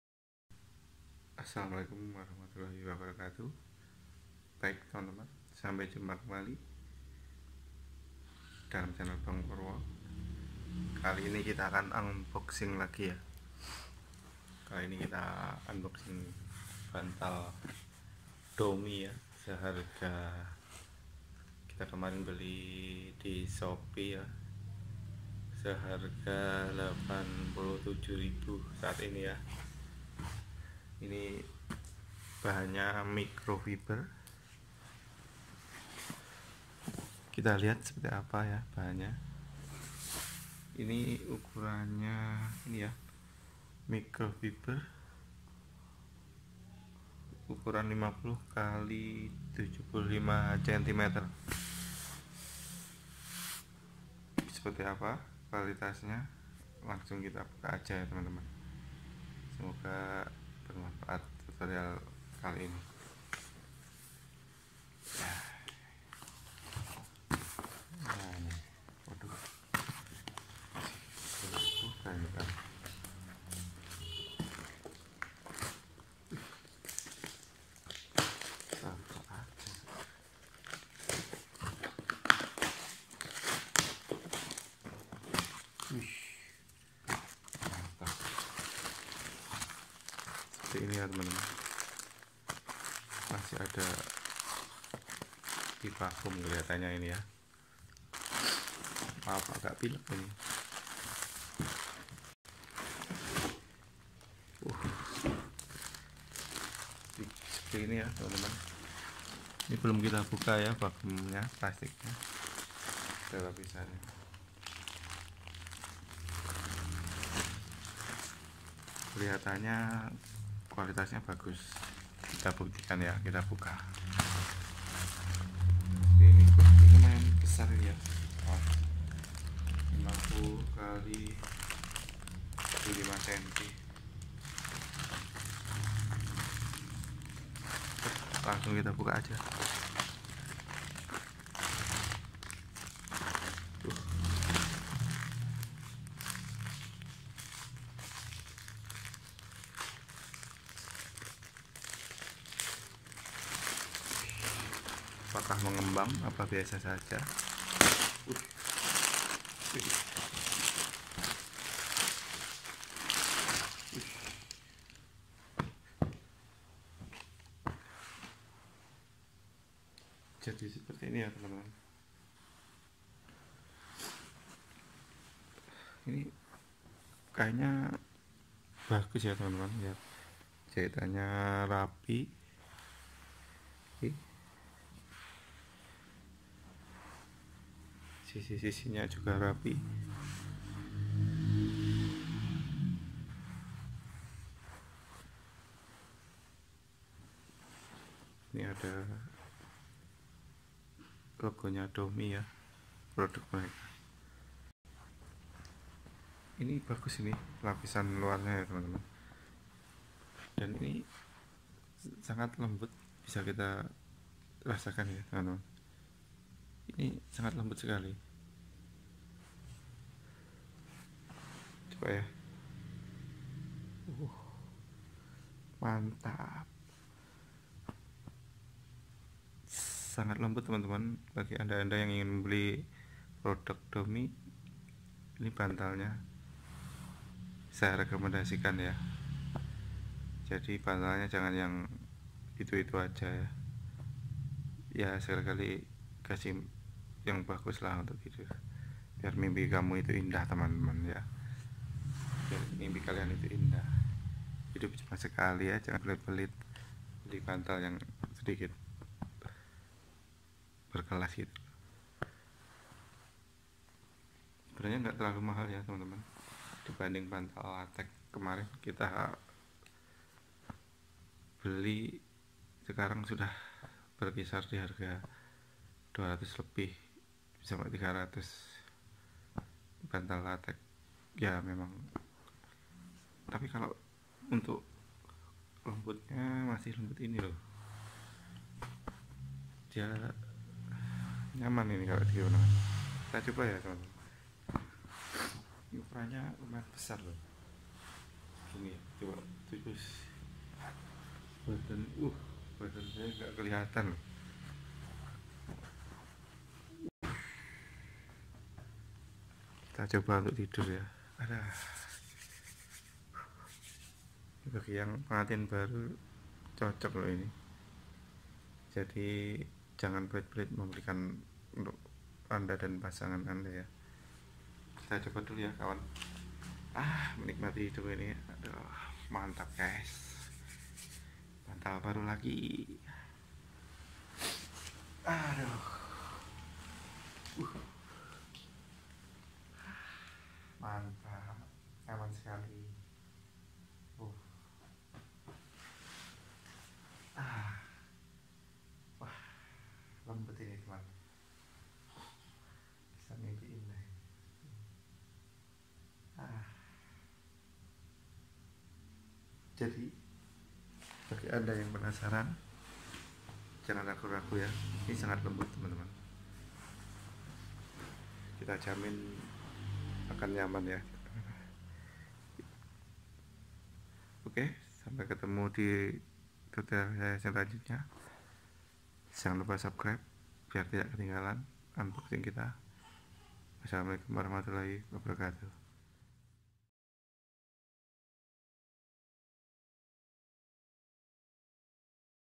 Unboxing Bantal DOMI ( BANTAL MICRO FIBER )
Bantal DOMI merupakan bantal yg terbuat dari bahan micro fiber yang empuk untuk di jadikan penyangga Kepala saat tidur. Lebih jelas ya simak video saya berikut ini